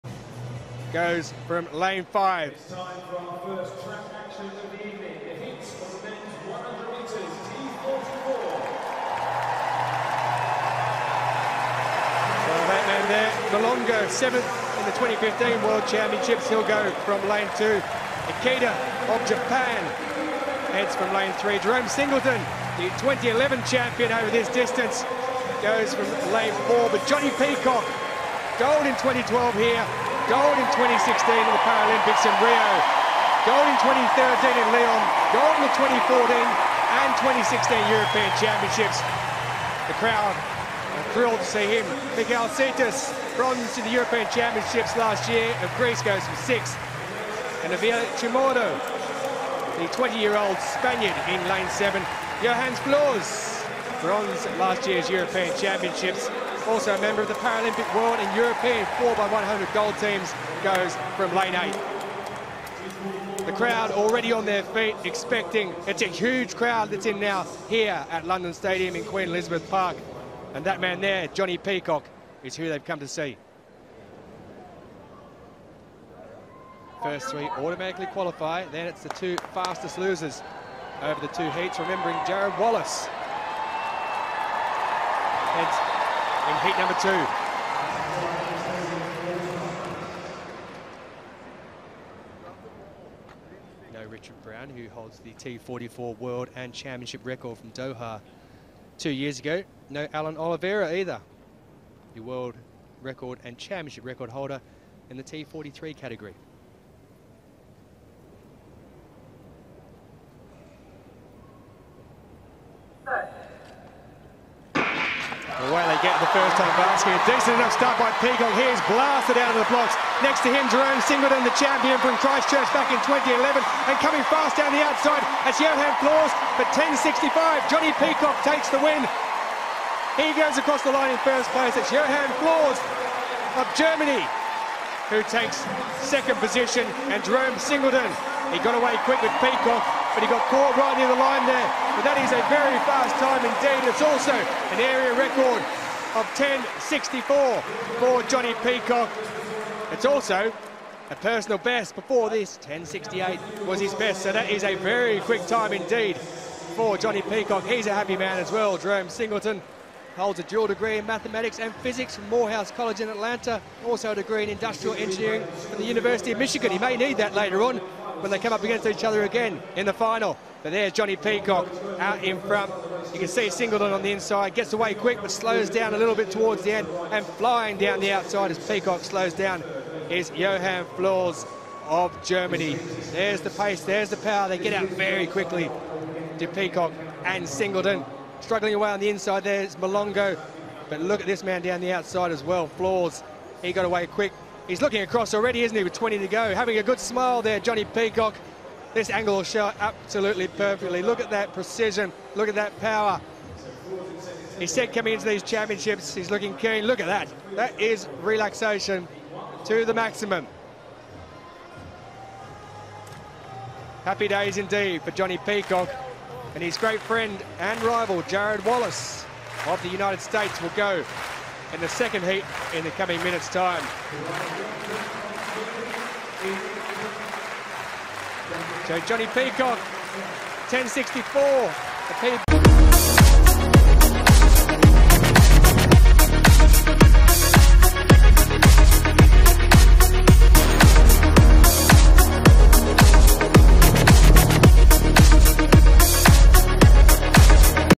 Men’s 100m T44 | Round 1 Heat 1 | London 2017 World Para Athletics Championships
Start list:
Mpumelelo MHLONGO South Africa
Mikio IKEDA Japan
Jerome SINGLETON United States of America
Jonnie PEACOCK Great Britain
Michail SEITIS Greece
Alberto CHAMORRO  AVILA Spain
Johannes FLOORS Germany